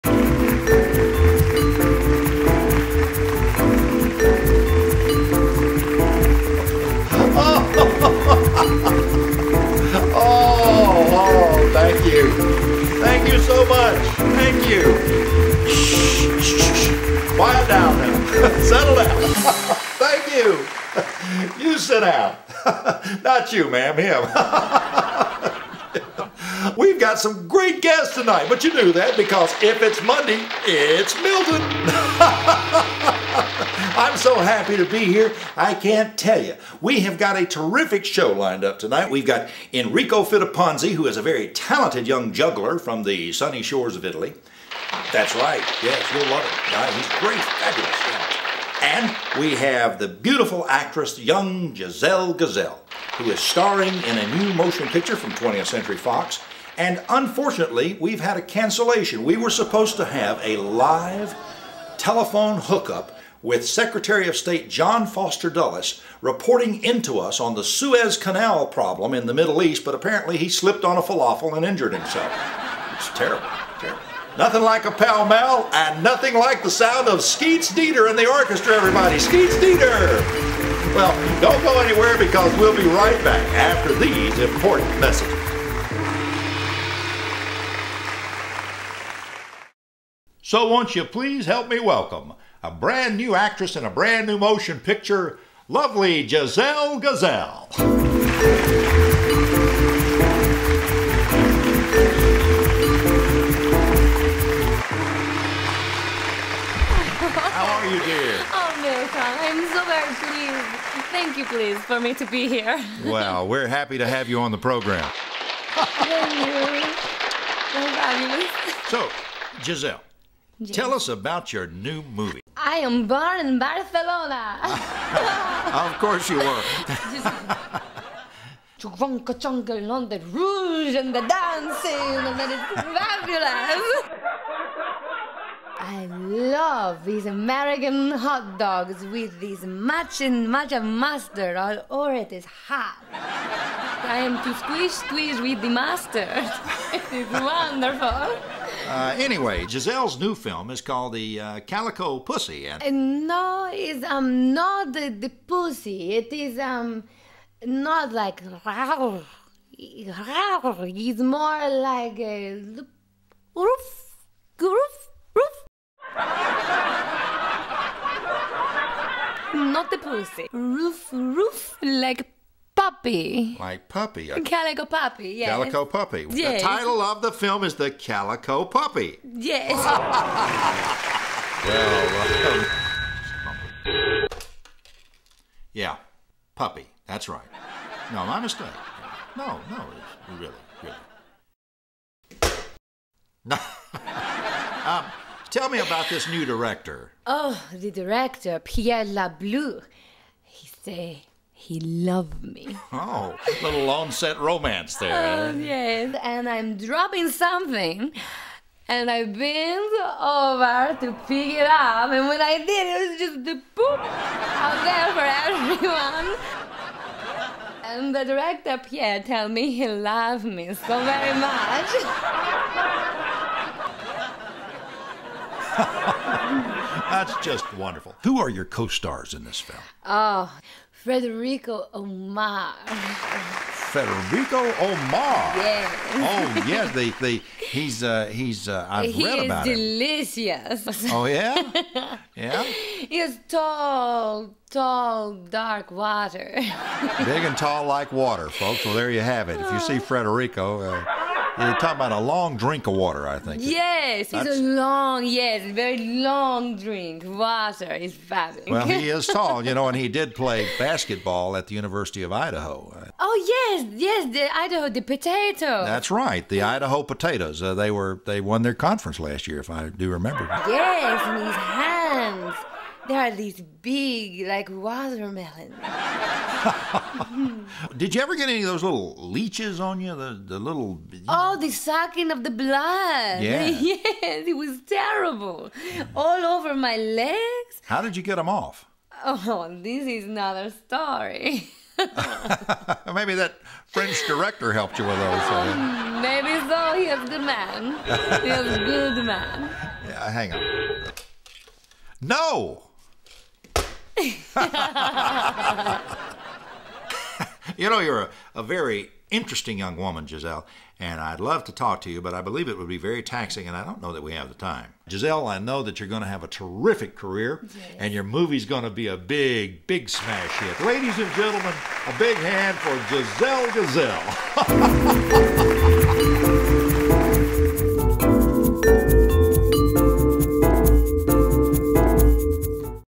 THE MILTON BAGBY 1950's TALK SHOW - Special guest Giselle Gazelle totally baffles Milton.
🎬 Tune in to THE MILTON BAGBY SHOW and join Alabama's favorite TV host Milton Bagby trying to interview wacko Spanish actress Giselle Gazelle.